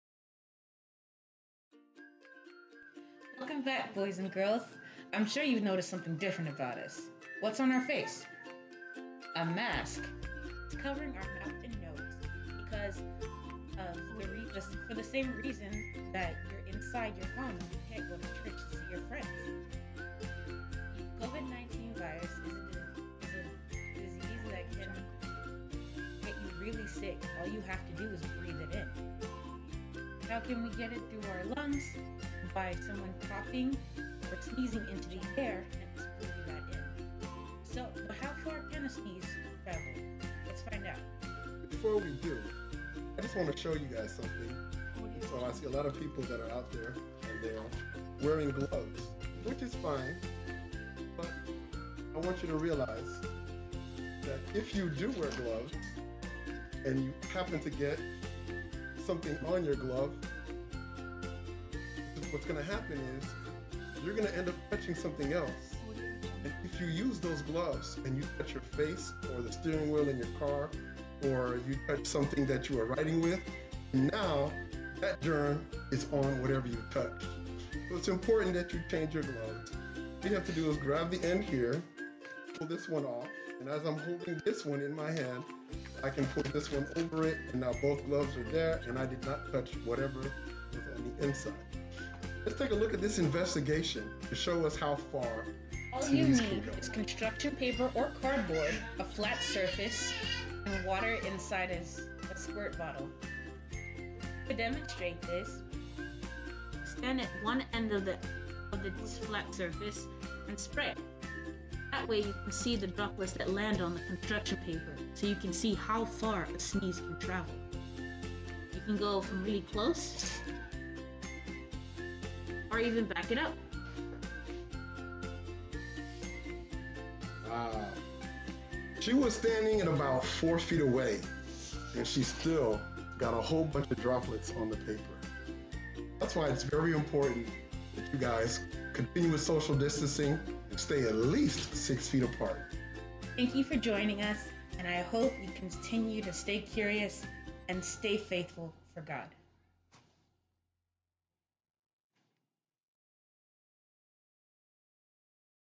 Northside SDA Church Children's Ministry: Science Corner w/Bro. Don Clerveaux, and Miss Jeannie G.
Northside SDA Church Children's Ministry: Science Corner with Bro. Don Clerveaux, and Miss Jeannie G.